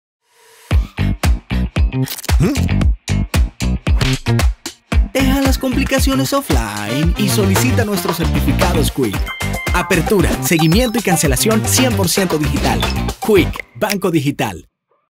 ¡Abre hoy tu Certificado Qik!
El que todavía hace filas o toma turnos para poner su dinero a crecer es porque quiere. 👀

Abre hoy tu Certificado Qik, calcula el rendimiento desde el principio, elige la opción que más te convenga y recibe tu certificado digital. ¡Todo en un instante, desde dondequiera que estés! 😎🤳